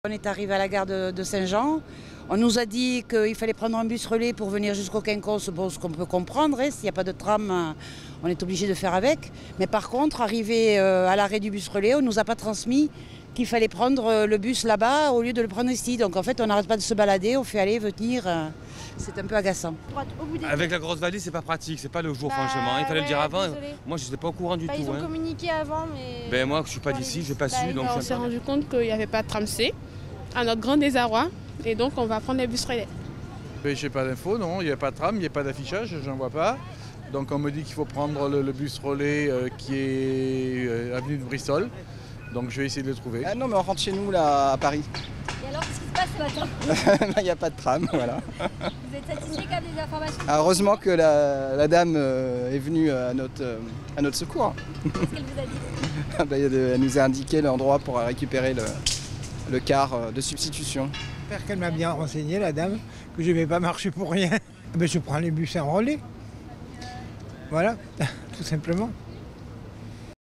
La ligne C du tramway interrompue
Pas de tramway sur la ligne C entre la station Grand-Parc et Gare Saint-Jean... Des travaux de maintenance en cours sur les rails ont laissé les usagers un peu perdus ce matin. Il a donc fallu s'adapter grâce aux différentes mesures mises en place et notamment des bus relais.